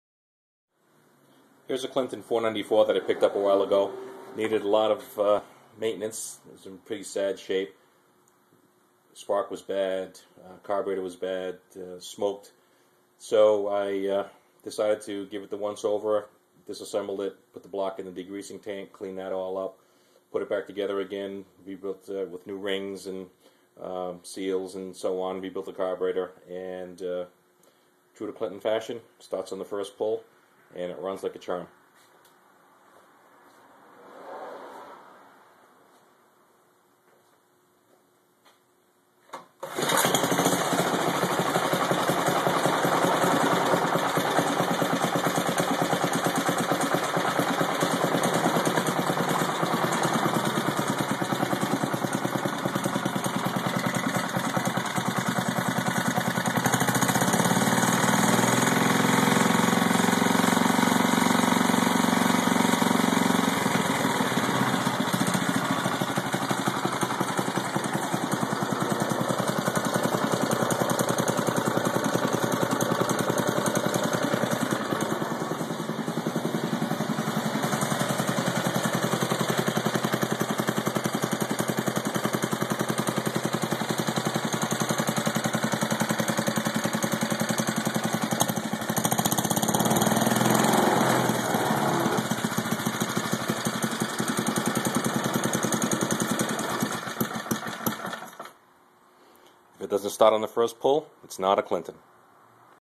Clinton engine, Model 494 (yet another) rebuild
Here's another of the many 494s that I have.  After about 40 years of seasonal commercial duty this engine was very tired, very dirty and a non-runner.  I originally just wanted to see if I could get it going again without too much effort.  I was successful.  However, never satisfied with just that, I alway went on to "just one more thing" until eventually the engine was completely torn down and sent to the degreasing tank and then to the media blast cabinet.  New rings (along with honing the cylinder), crankshaft seals and gaskets were utilized, as was some paint.  The short block now appears new.  The blower housing, cooling fins shroud and gas tank bracket are untouched at this point but they'll be re-done too.  These engines are simply workhorses.  They're cast iron, so simple and thoroughly reliable.  I'd be hard pressed to believe you'll find those cheap engines manufactured in China in usable condition 40 or 50 years from now.